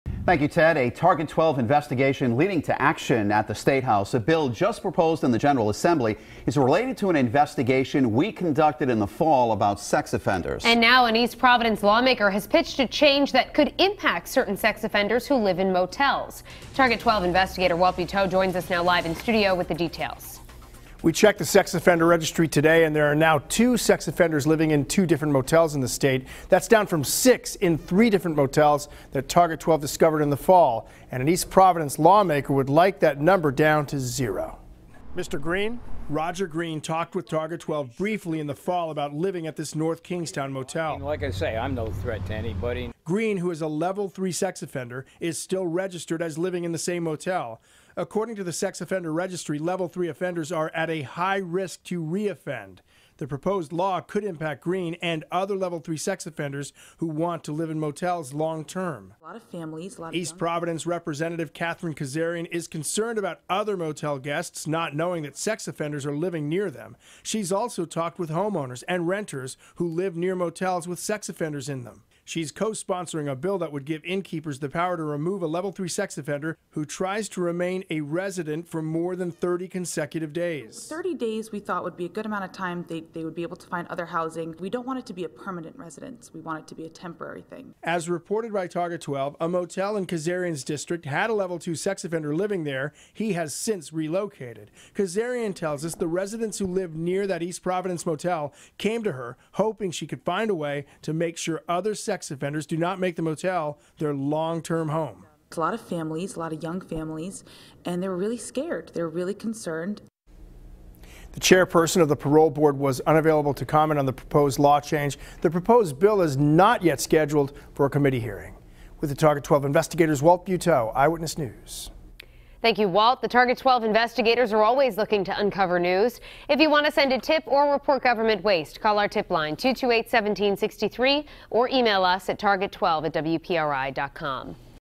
Law Would Bar High-Risk Sex Offenders from Living in Hotels
The bill was introduced by Rep. Katherine Kazarian, D-East Providence.